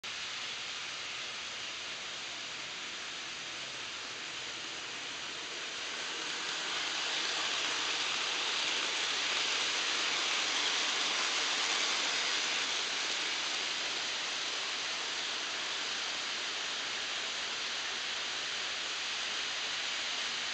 Kansas City Southern trains passing in HO scale
Older passenger F3's power one train while newer AC4400's lead the other.